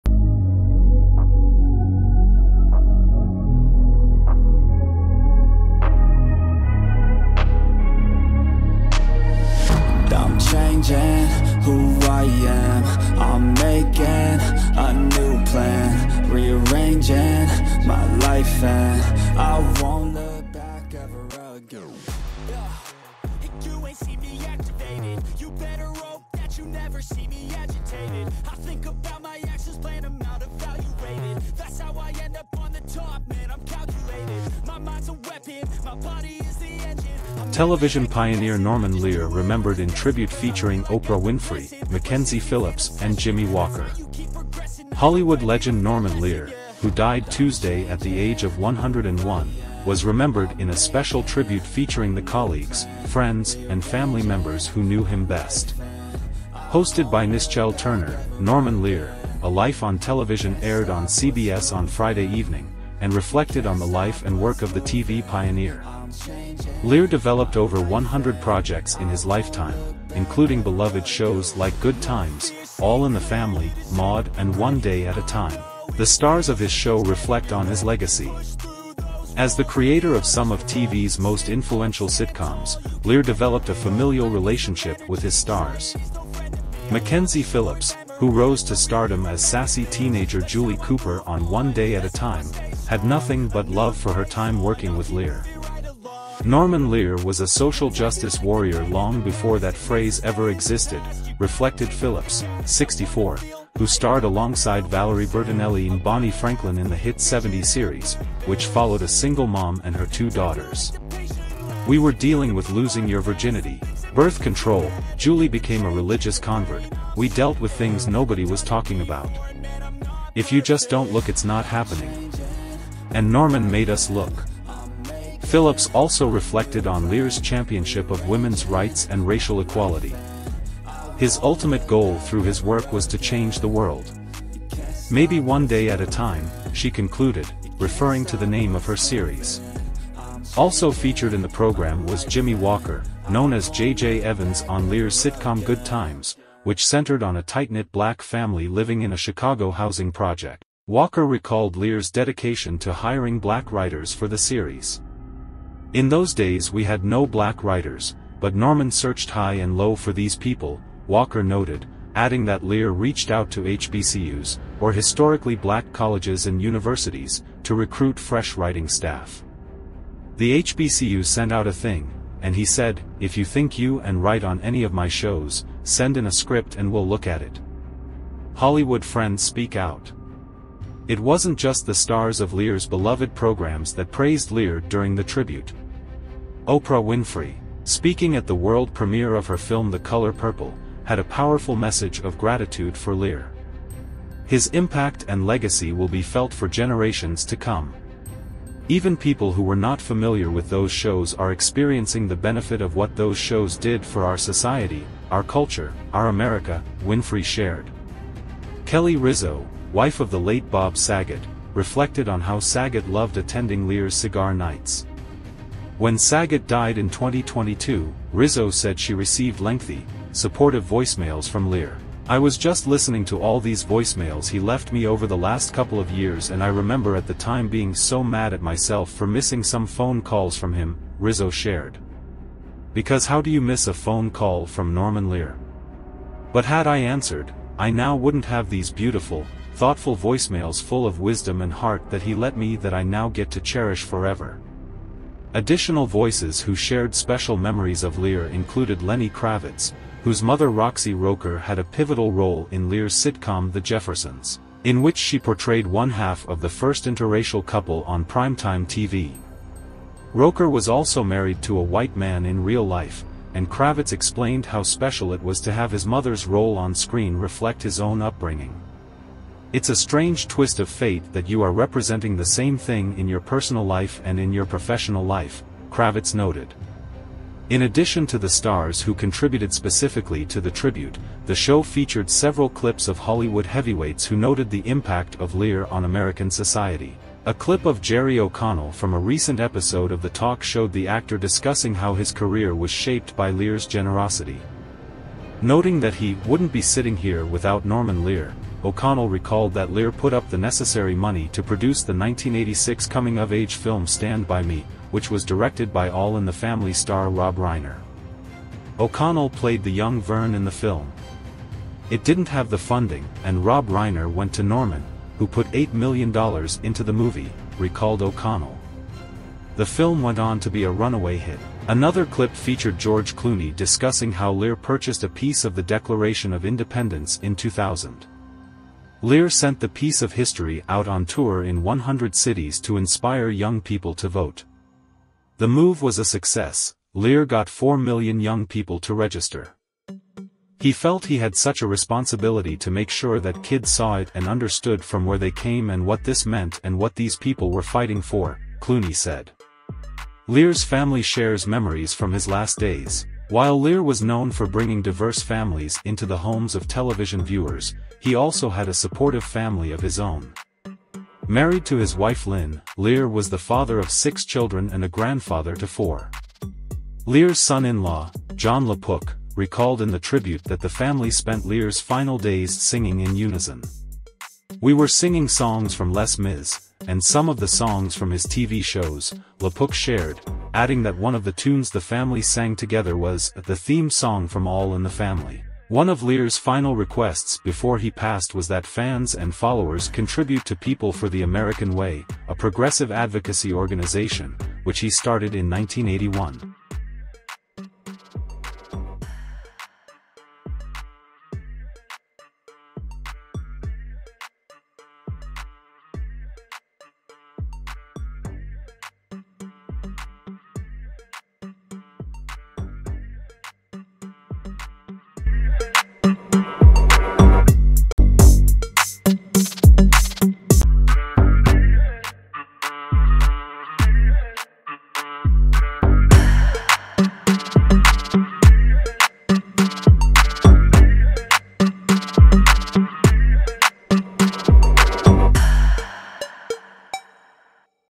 Television pioneer Norman Lear remembered in tribute featuring Oprah Winfrey, Mackenzie Phillips and
Follow our YouTube channel to get the latest news from the world
Television pioneer Norman Lear remembered in tribute featuring Oprah Winfrey, Mackenzie Phillips and Jimmie Walker

Hollywood legend Norman Lear, who died Tuesday at the age of 101, was remembered in a special tribute featuring the colleagues, friends and family members who knew him best. Hosted by Nischelle Turner, Norman Lear: A Life on Television aired on CBS on Friday evening, and reflected on the life and work of the TV pioneer. Lear developed over 100 projects in his lifetime, including beloved shows like Good Times, All in the Family, Maude and One Day at a Time.
The stars of his show reflect on his legacy
As the creator of some of TV's most influential sitcoms, Lear developed a familial relationship with his stars. Mackenzie Phillips, who rose to stardom as sassy teenager Julie Cooper on One Day at a Time, had nothing but love for her time working with Lear.
"Norman Lear was a social justice warrior long before that phrase ever existed," reflected Phillips, 64, who starred alongside Valerie Bertinelli and Bonnie Franklin in the hit '70s series, which followed a single mom and her two daughters. "We were dealing with losing your virginity, birth control, Julie became a religious convert, we dealt with things nobody was talking about. 'If you just don't look it's not happening.' And Norman made us look."
Phillips also reflected on Lear's championship of women's rights and racial equality. "His ultimate goal through his work was to change the world. Maybe one day at a time," she concluded, referring to the name of her series.
Also featured in the program was Jimmie Walker, known as J.J. Evans on Lear's sitcom Good Times, which centered on a tight-knit Black family living in a Chicago housing project. Walker recalled Lear's dedication to hiring Black writers for the series.
"In those days we had no Black writers, but Norman searched high and low for these people," Walker noted, adding that Lear reached out to HBCUs, or historically black colleges and universities, to recruit fresh writing staff. "The HBCUs sent out a thing, and he said 'If you think you and write on any of my shows, send in a script and we’ll look at it."
Hollywood friends speak out
It wasn't just the stars of Lear's beloved programs that praised Lear during the tribute. Oprah Winfrey, speaking at the world premiere of her film The Color Purple, had a powerful message of gratitude for Lear.
"His impact and legacy will be felt for generations to come. Even people who were not familiar with those shows are experiencing the benefit of what those shows did for our society, our culture, our America," Winfrey shared.
Kelly Rizzo, wife of the late Bob Saget, reflected on how Saget loved attending Lear's cigar nights. When Saget died in 2022, Rizzo said she received lengthy, supportive voicemails from Lear.
"I was just listening to all these voicemails he left me over the last couple of years and I remember at the time being so mad at myself for missing some phone calls from him," Rizzo shared. "Because how do you miss a phone call from Norman Lear? But had I answered, I now wouldn’t have these beautiful, thoughtful voicemails full of wisdom and heart that he let me that I now get to cherish forever."
Additional voices who shared special memories of Lear included Lenny Kravitz, whose mother Roxie Roker had a pivotal role in Lear's sitcom The Jeffersons, in which she portrayed one-half of the first interracial couple on primetime TV. Roker was also married to a white man in real life, and Kravitz explained how special it was to have his mother's role on screen reflect his own upbringing.
"It’s a strange twist of fate that you are representing the same thing in your personal life and in your professional life," Kravitz noted.
In addition to the stars who contributed specifically to the tribute, the show featured several clips of Hollywood heavyweights who noted the impact of Lear on American society. A clip of Jerry O'Connell from a recent episode of The Talk showed the actor discussing how his career was shaped by Lear's generosity. Noting that he "wouldn't be sitting here without Norman Lear," O'Connell recalled that Lear put up the necessary money to produce the 1986 coming-of-age film Stand By Me, which was directed by All in the Family star Rob Reiner. O'Connell played the young Vern in the film.
"It didn’t have the funding, and Rob Reiner went to Norman, who put $8 million into the movie," recalled O'Connell. The film went on to be a runaway hit.
Another clip featured George Clooney discussing how Lear purchased a piece of the Declaration of Independence in 2000. Lear sent the piece of history out on tour in 100 cities to inspire young people to vote. The move was a success: Lear got 4 million young people to register.
"He felt he had such a responsibility to make sure that kids saw it and understood from where they came and what this meant